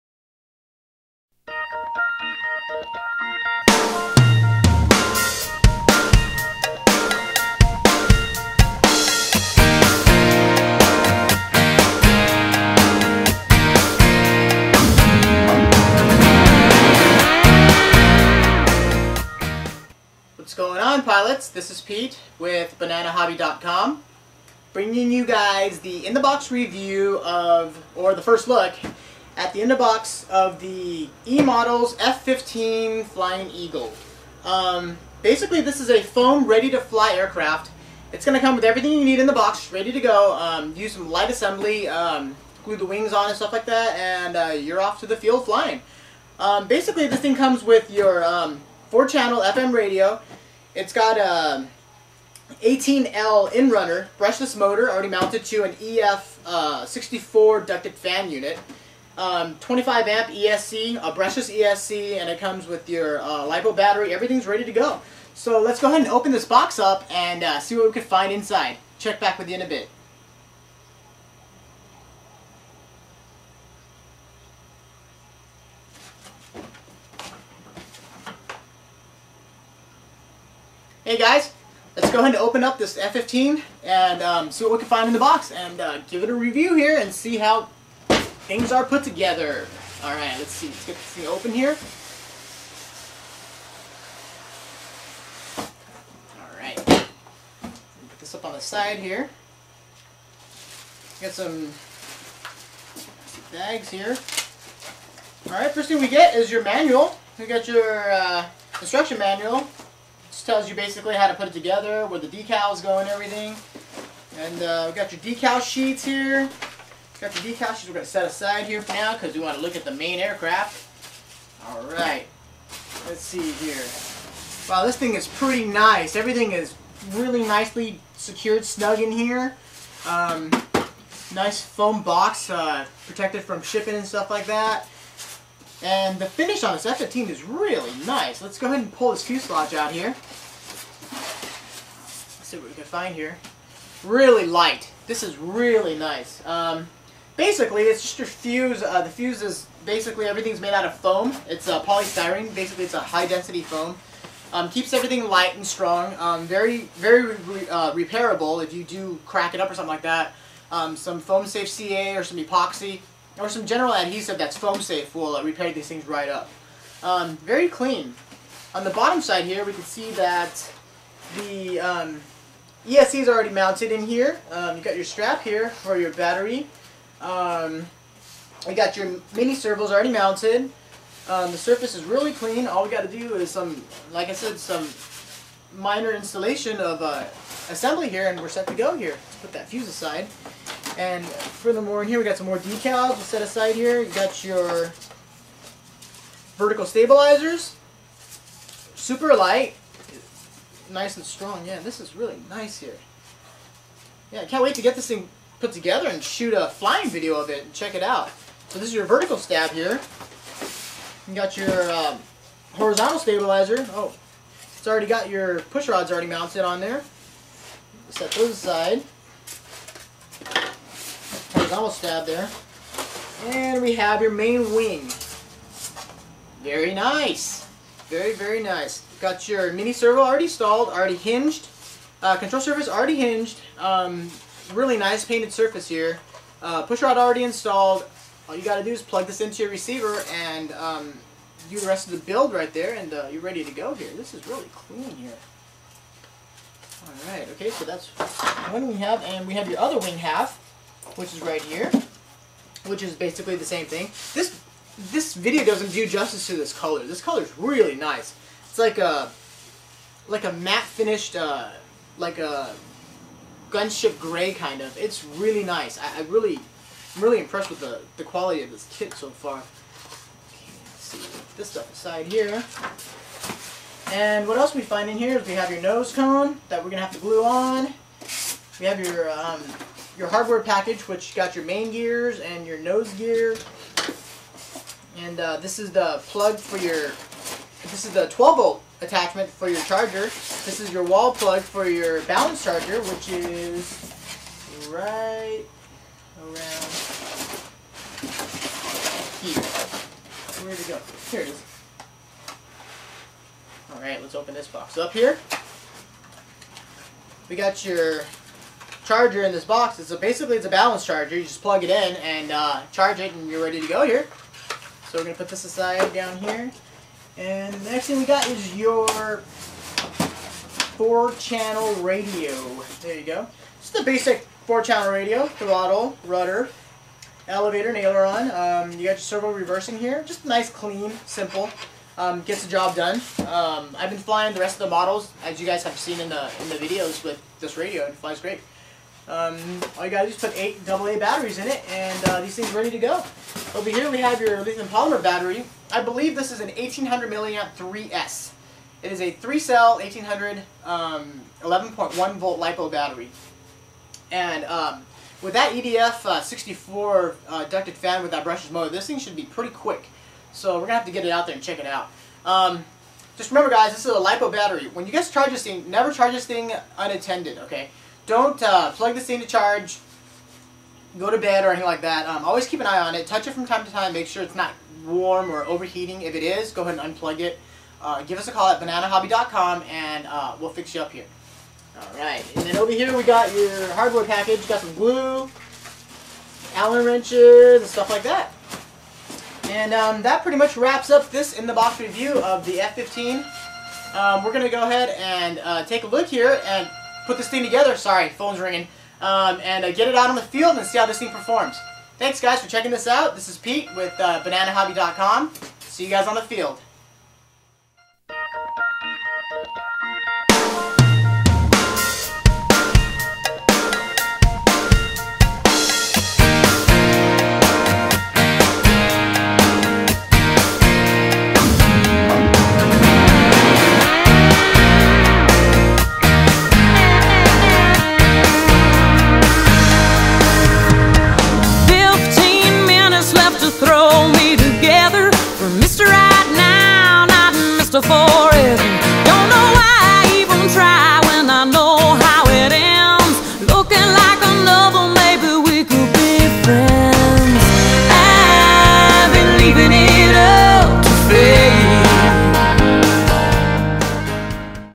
F-15 Eagle RTF Ducted Fan Jet! What's in the Box Review!
A look at whats in the box at the bananahobby.com F-15 Eagle RTF!